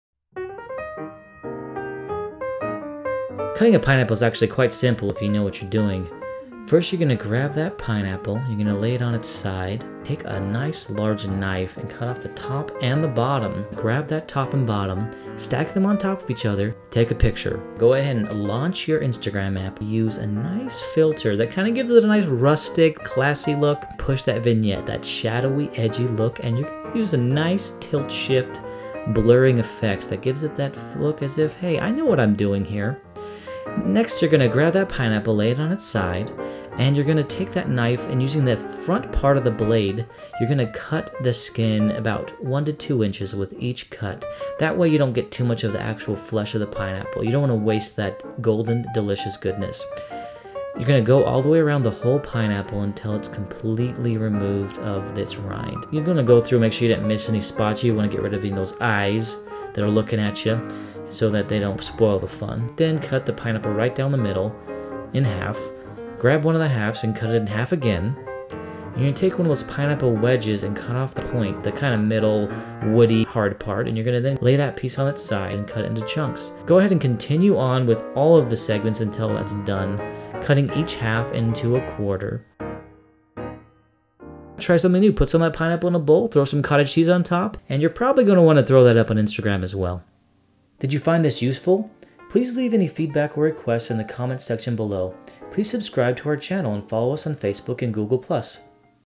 How to Cut a Pineapple Like an Adult
Cutting a Pineapple is easy . . . IF you know what you are doing. Follow these steps and you will look like a pro, and your whole family will enjoy!